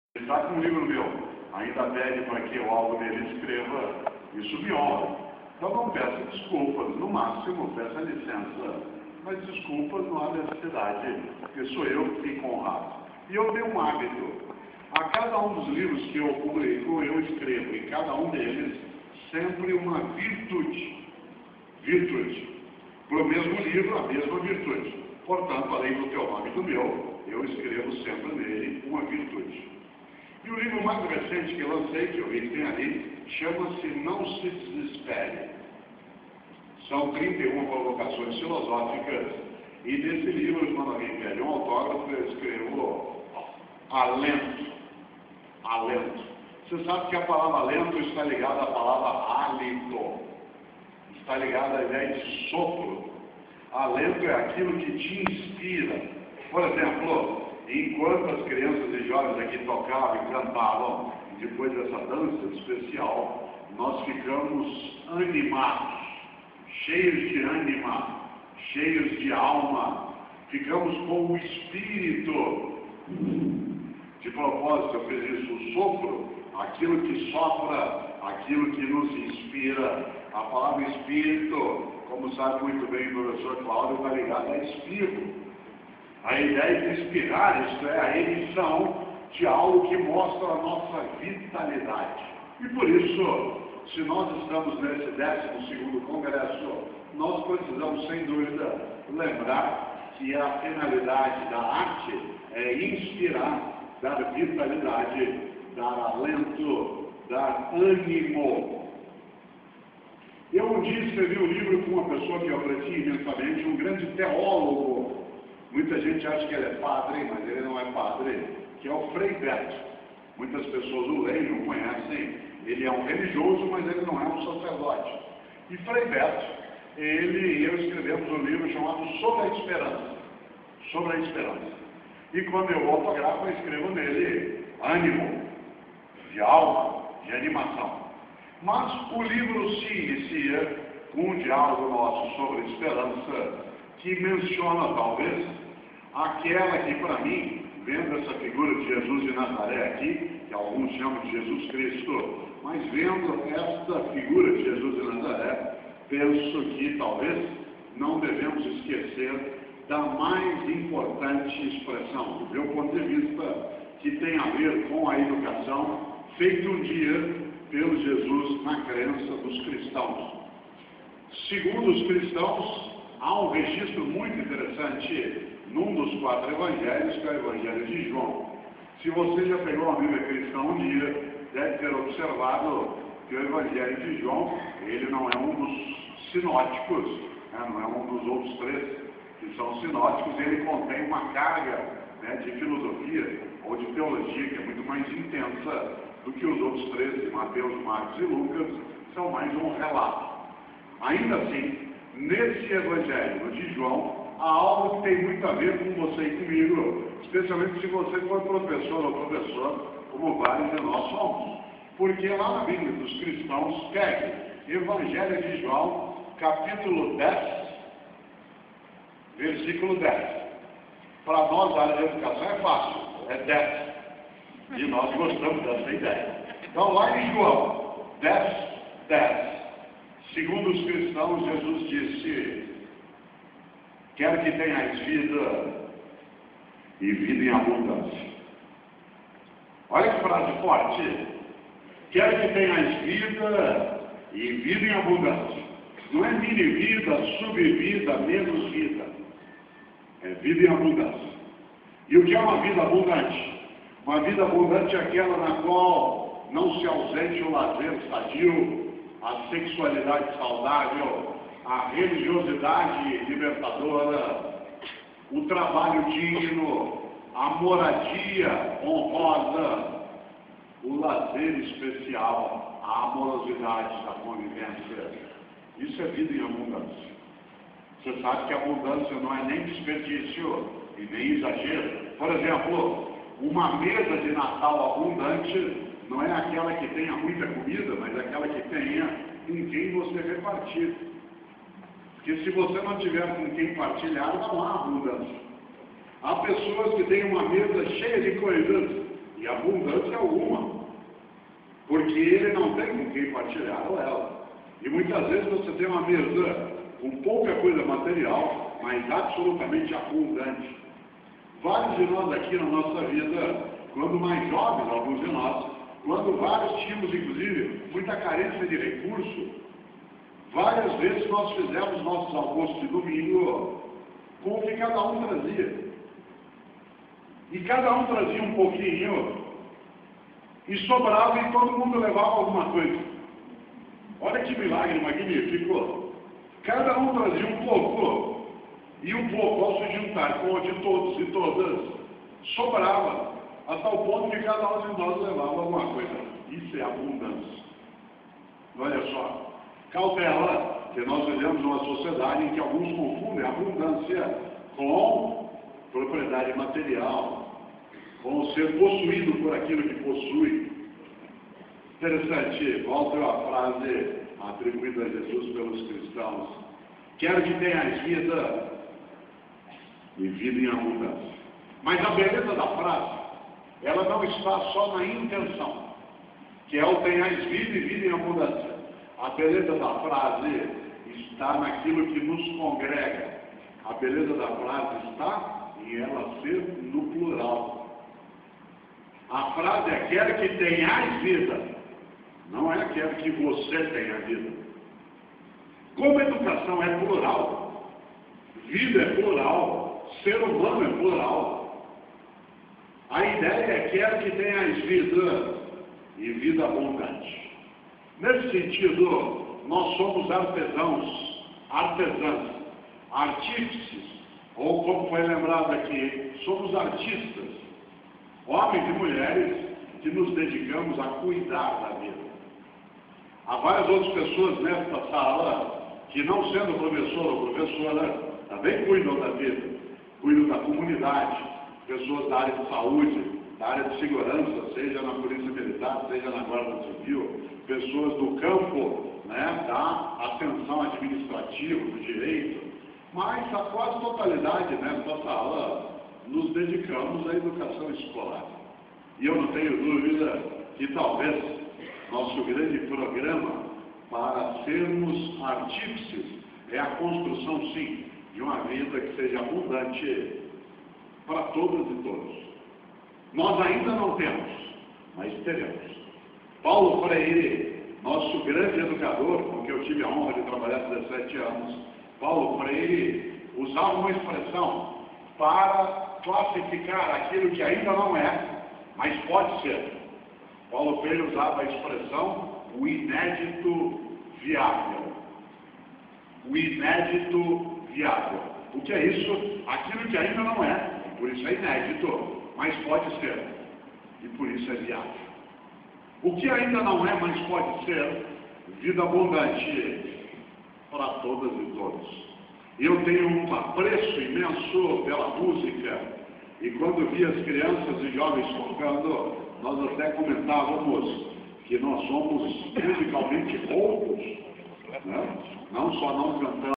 TV PORTAL - PAlestra Prof. Cortella 12º Congresso Int. de Educação LBV 2/5
Palestra do Professor Mario Sergio Cortella na abertura do 12º Congresso Internacional de Educação LBV no Bom Retiro
26/06/13